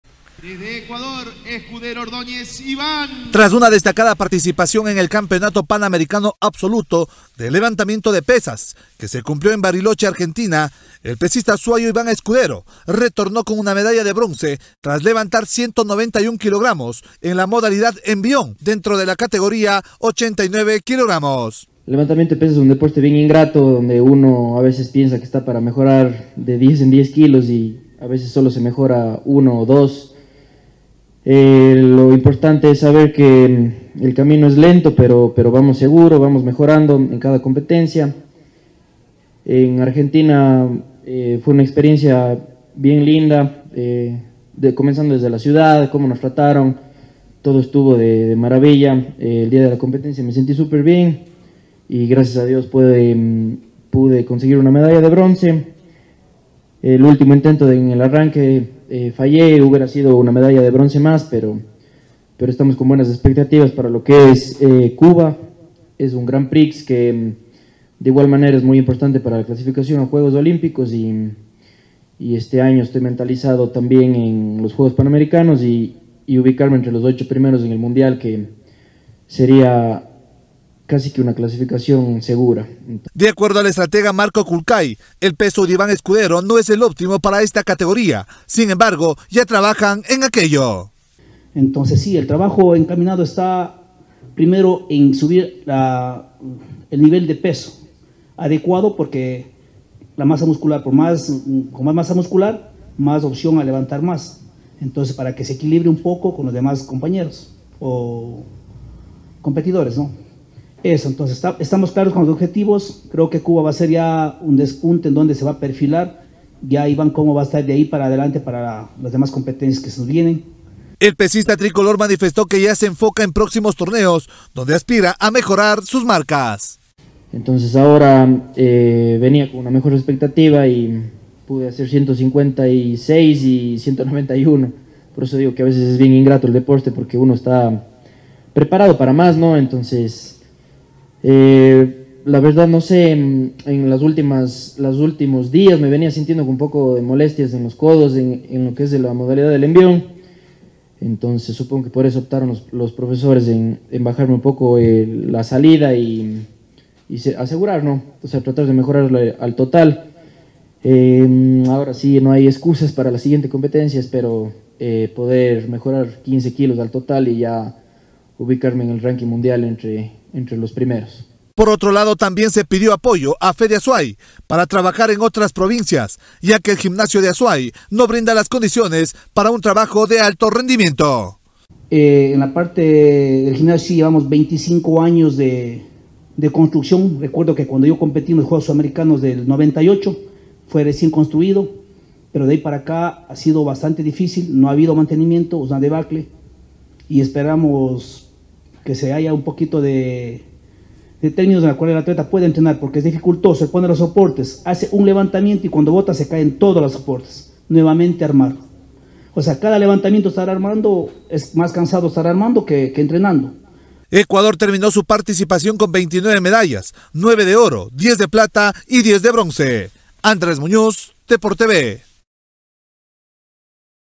Iván Escudero retornó con presea de bronce del panamericano de Halterofilia
Tras una destacada participación en  el campeonato panamericano absoluto de levantamiento de pesas, que se cumplió en Argentina, el pesista azuayo, Iván Escudero, retorno con una medalla de bronce tras levantar 191 kg en la modalidad envión  dentro de la categoría 89kg…